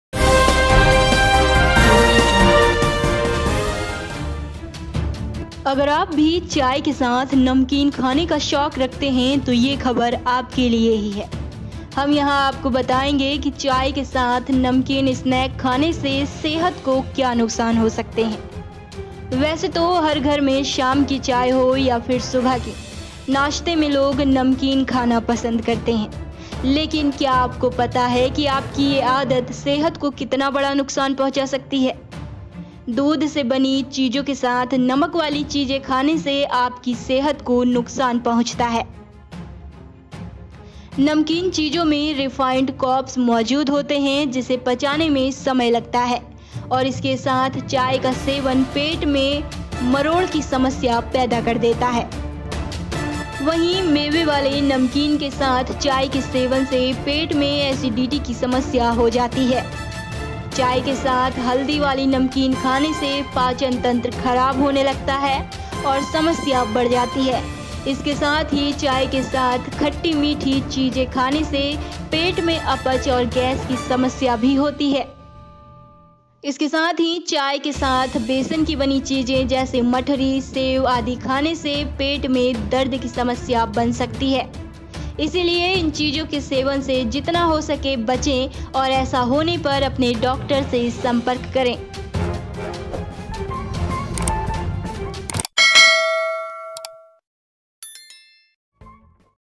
Teawithsnacks : अगर आप भी है chailover ... तो ये खबर जानकर हो जाएंगे हैरान ||ibharattv
आप भी अगर चाय के साथ नमकीन खाने का शौक रखते हैं तो यह खबर आप ही के लिए है. हम यहां आपको बताएंगे कि चाय के साथ नमकीन स्नैक खाने के सेहत को क्या नुकसान हो सकते हैं. वैसे तो हर घर में शाम की चाय हो या फिर सुबह की...नाश्ते में लोग नमकीन खाना पसंद करते हैं. लेकिन क्या आपको पता है कि आपकी ये आदत सेहत को कितना बड़ा नुकसान पहुंचा सकती है.